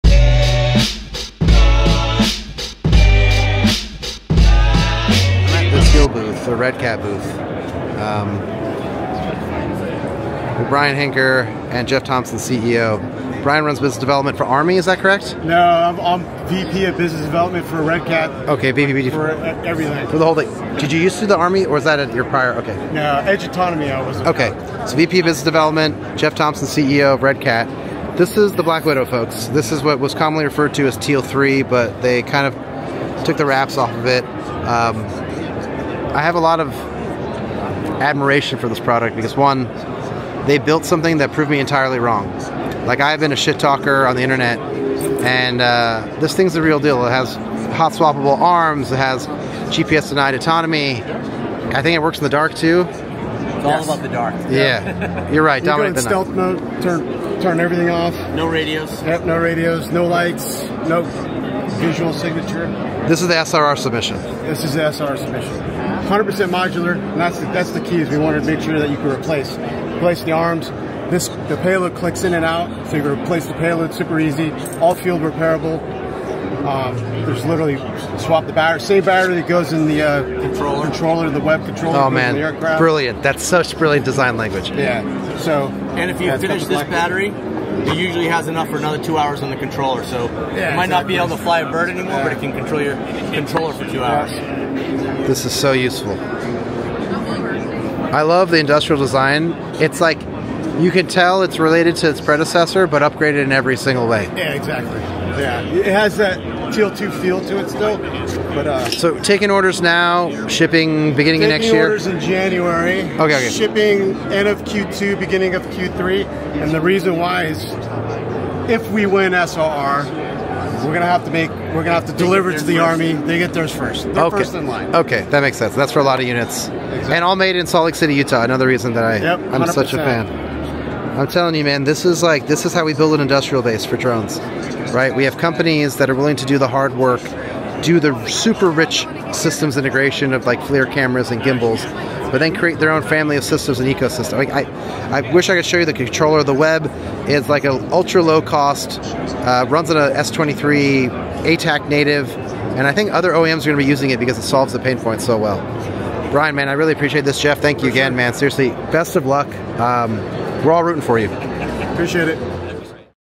AUSA24 - Teal Drones Black Widow
The Redcat/Teal executive team shows us the Black Widow (formally known as the Teal 3) short range reconnaissance sUAS platform.

It's uniquely modular and purpose built for the warfighter - also built in Salt Lake City, Utah.